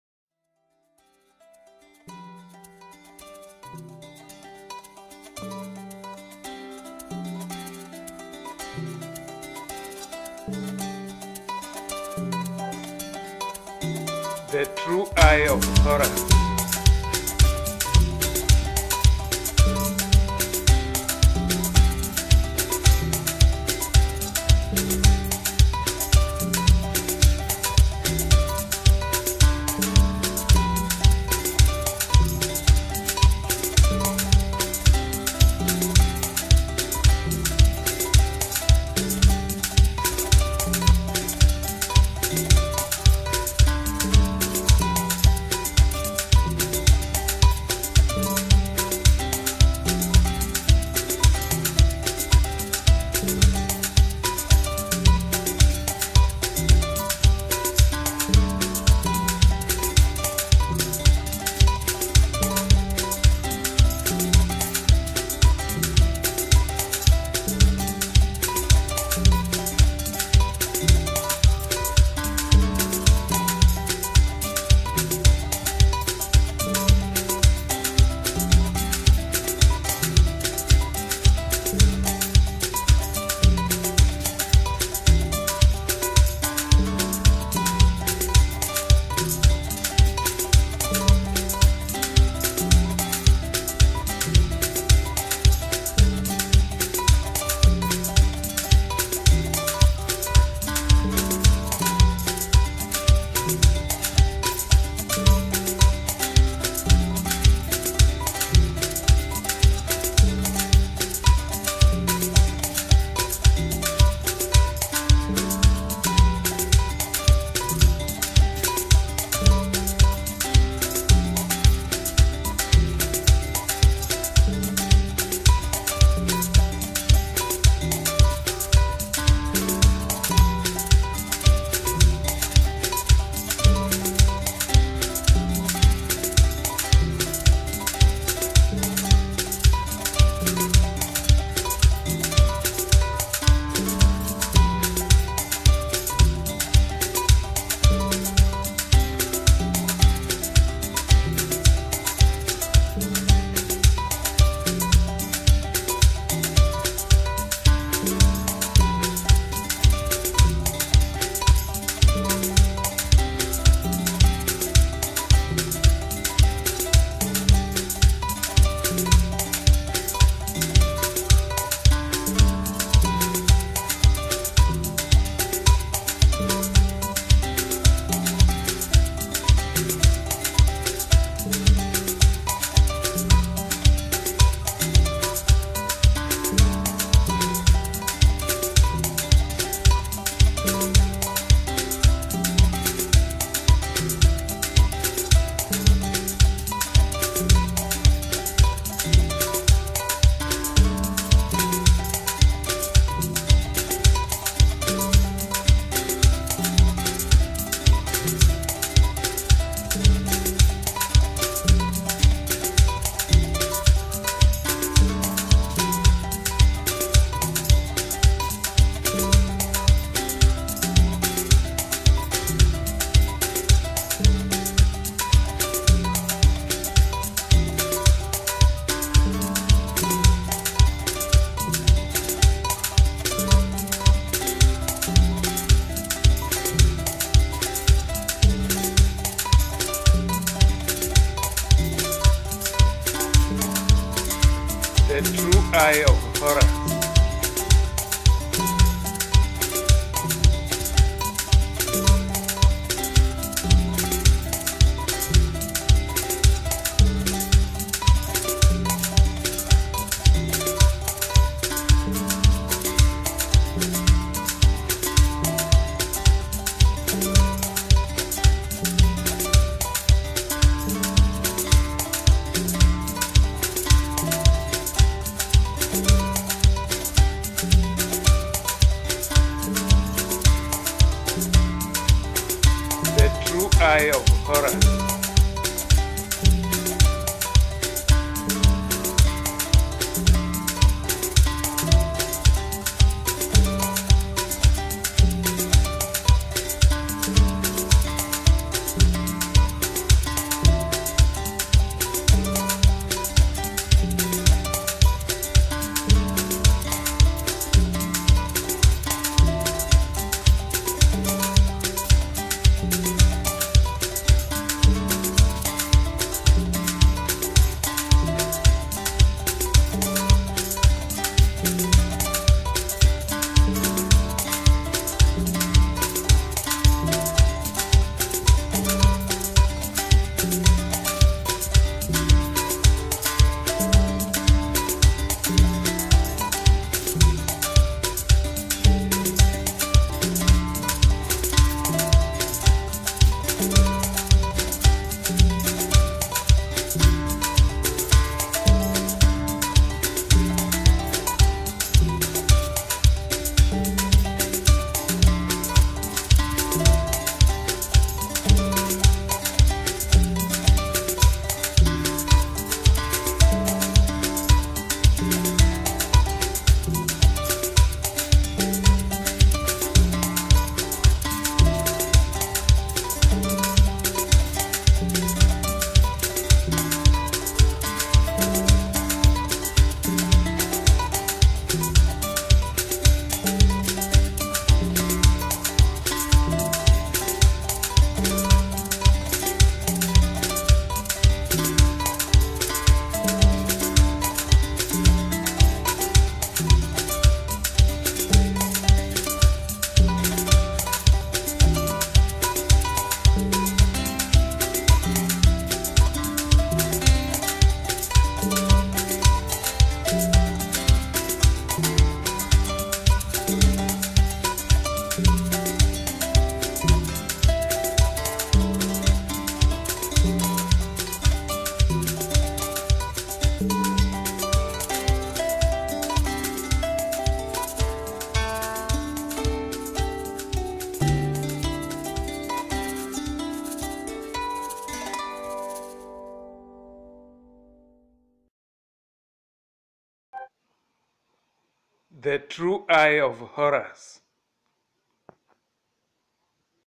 The Eye of Horus
The Eye of Horus is a sculpture by Fine Artist Odhiambo Siangla, PhD from Kenya, East Africa. He practices art in Pennsylvania, U.S.A. In this piece Odhiambo expresses understanding of the basic principles of American democracy. According to this artist, The Horus Eye exhibits right to life, liberty, and pursuit of happiness. He believes that the piece tells about responsibility for the common good; equality of opportunity and equal protection of the law, freedom of speech and religion; majority rule with protection of minority rights.